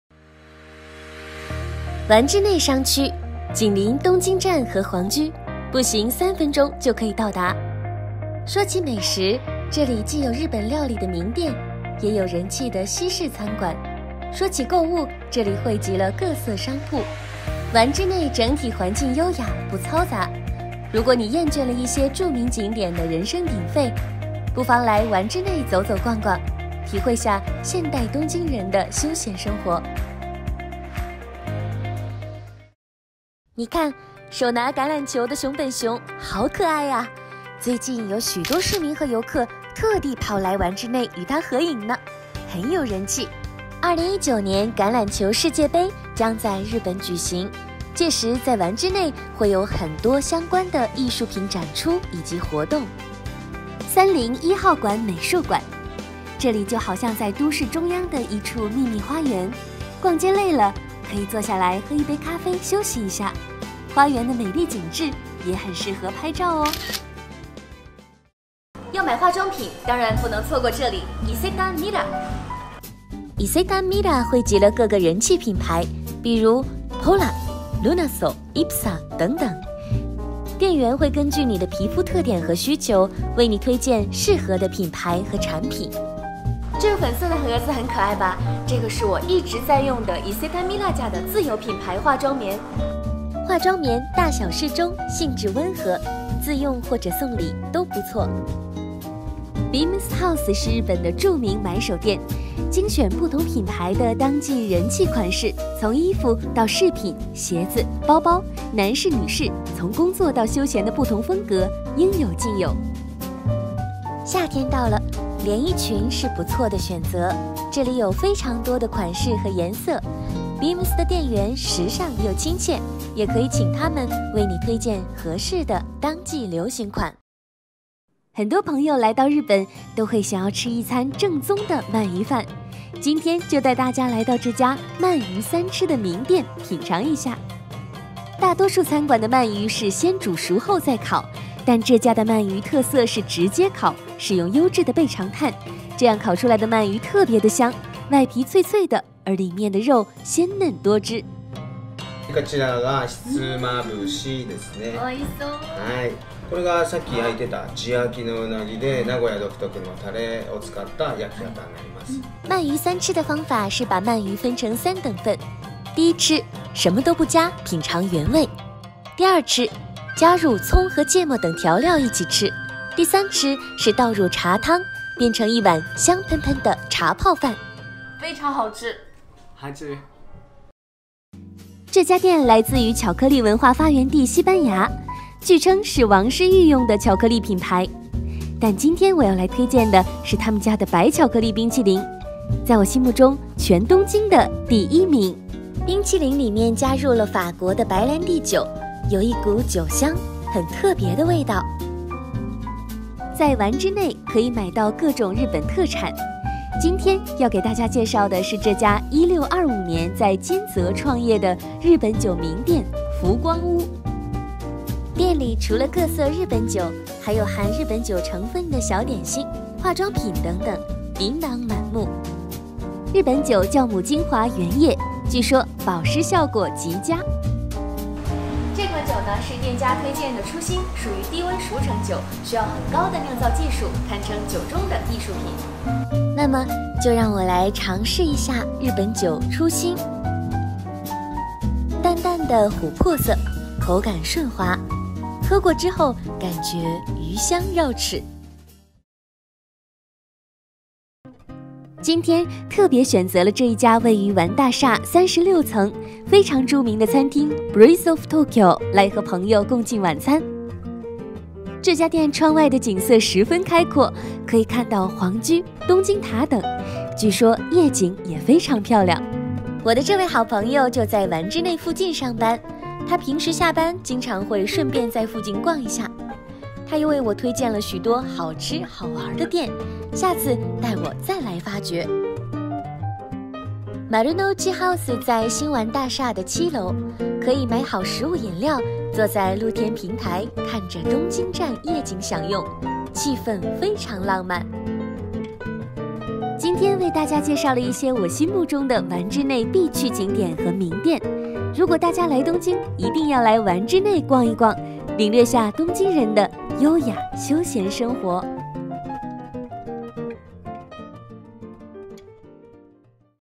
丸之内攻略／丸の内攻略
丸之内毗邻东京站和皇居，有购物，有美食，优雅不嘈杂的环境，浪漫的氛围，可谓一日观光的好去处！跟着视频来做攻略吧！

東京駅と皇居のすぐ近くにある丸の内は、ショッピングスポット、グルメスポットがそろっていて、エレガント、かつ、ロマンチックな雰囲気で一日を過ごすのに最適な場所！動画を見て攻略しましょう！

丸の内の詳しい情報は、こちら。